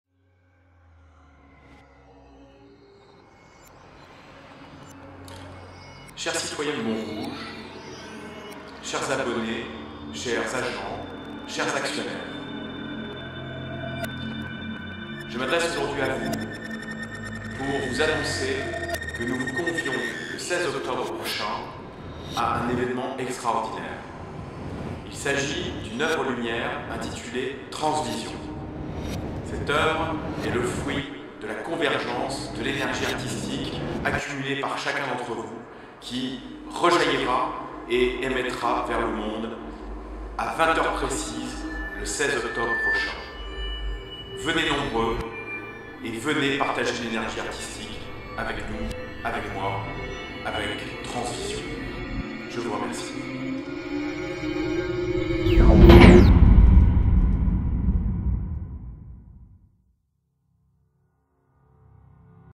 TRANSVISION : Le 16 octobre 2013 à 20h au Beffroi de Montrouge
"Montrougiennes, Montrougiens, je prends possession de votre Beffroi le 16 octobre de 20h à 20h30"

TRANSvision, oeuvre-lumière de Yann Toma,
invité d'honneur de la JCE
Transmission son et lumière sur le Beffroi, le mercredi 16 octobre de 20h à 20h30, place Cresp. Suivi du vernissage nocturne de la Biennale Jeune Création Européenne de 20h30 à minuit

Yann Toma
Ouest-Lumière sur 92120.fr
Accès libre